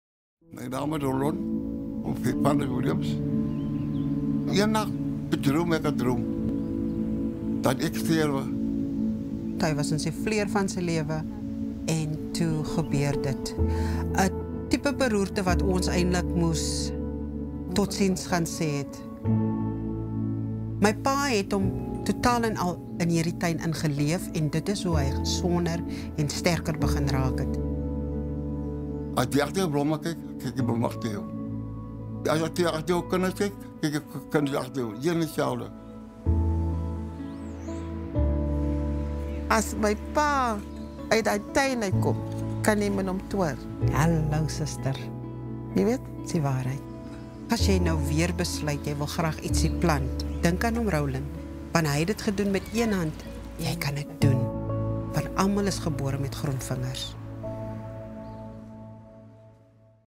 "Gardening saved his life"
In the prime of his life, Rowland Williams suffered a devastating stroke. Gardening has given him the space to regain his health and rebuild his strength one plant at a time.

Over the past two decades, Rowland has spent his days taking care of his plants. And, in turn, his plants have taken care of him!

Join (Oom) Rowland and celebrate the green spaces that heal and bring you joy this Garden Day.